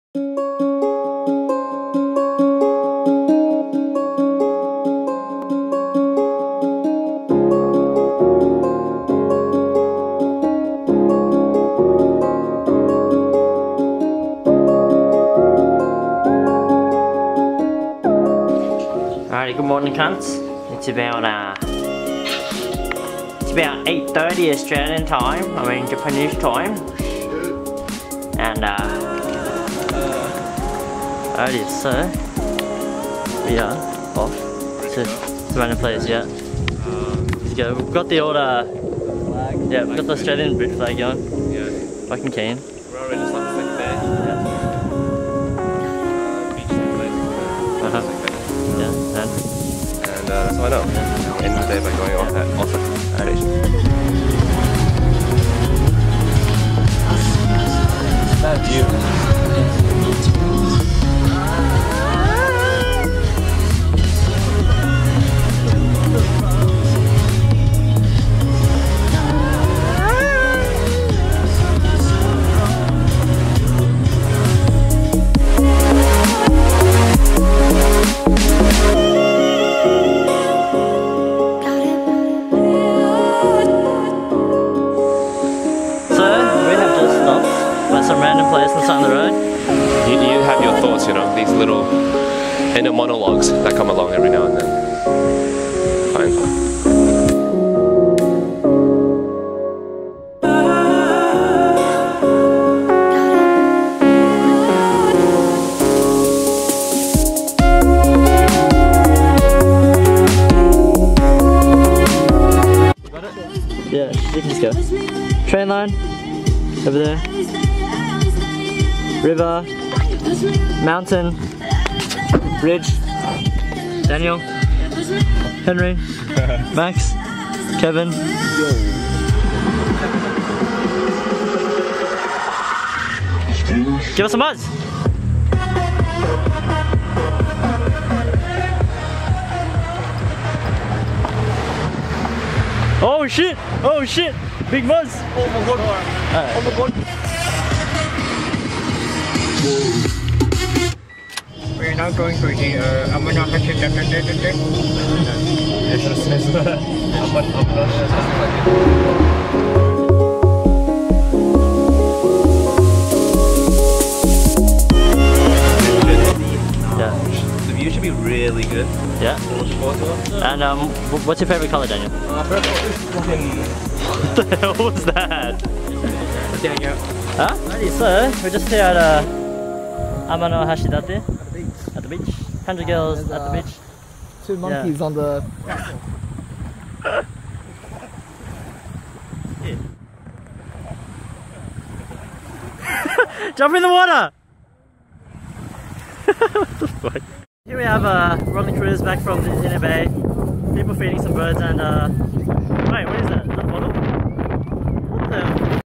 JAPAN VLOG 6 [JULY 2016]: Amanohashidate + Driving(天橋立＋運転する）
In this vlog, on the 12th of July we finally leave Kinosaki Onsen luggage in tow and drive towards Amanohashidate where we were met with some poor weather unfortunately, thus the lack of footage.

Song: